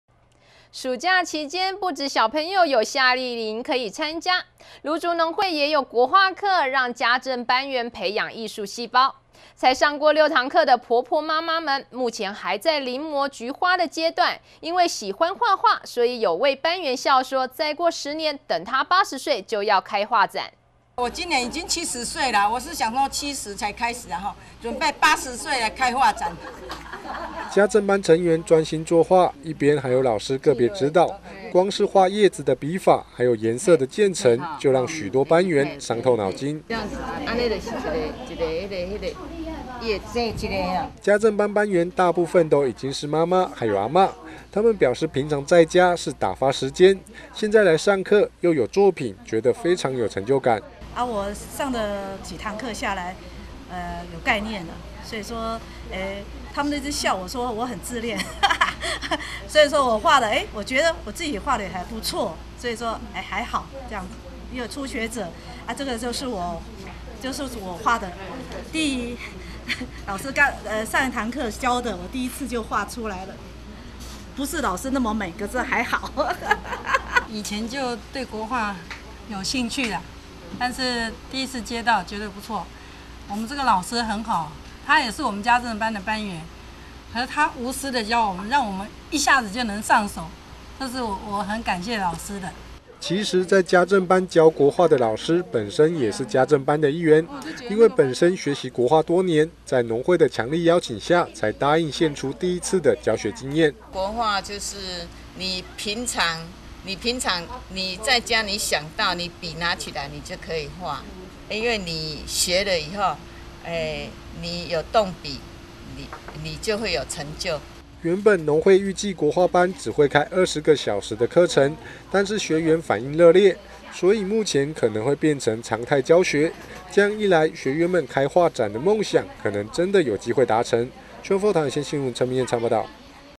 桃園有線新聞1010803-婆婆媽媽學國畫 笑說80歲開畫展
暑假期間，不只小朋友有夏令營可以參加。蘆竹農會也有國畫課，讓家政班員培養藝術細胞。才上過六堂課的婆婆媽媽們，目前還在臨摹菊花的階段。因為喜歡畫畫，所以有位班員笑說，再過10年等他八十歲就要開畫展。